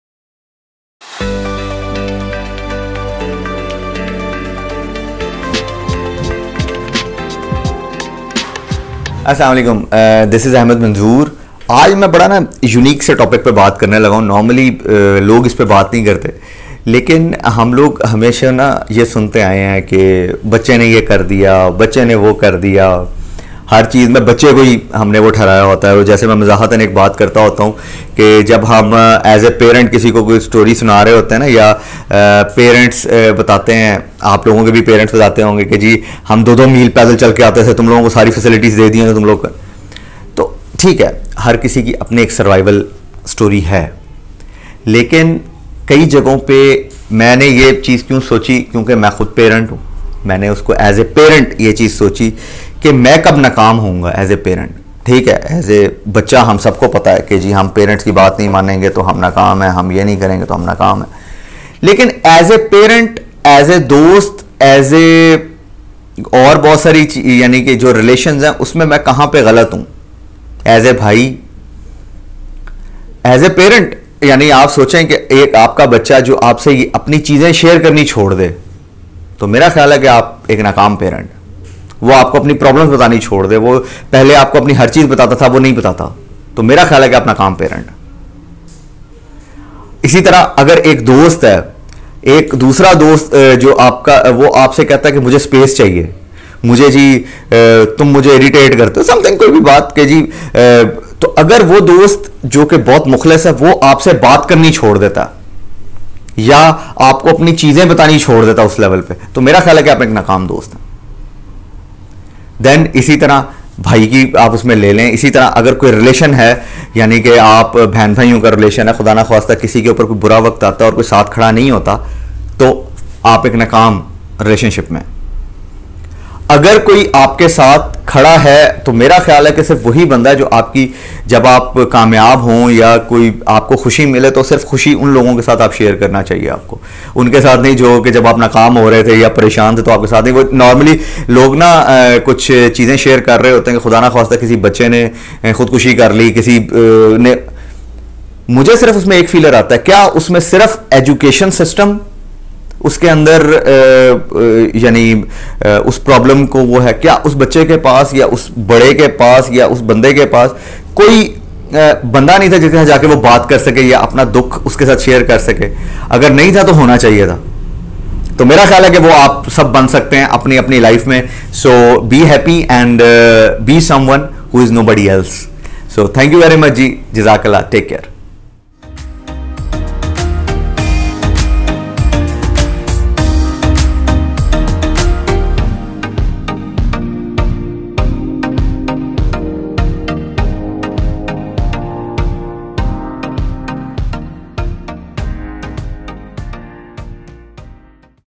Are we failures, this is my thought | Ahmad Manzoor
This is just my thought as a parent, friend, brother or a relative, that "Are we Failures".

Please help, listen & support each other.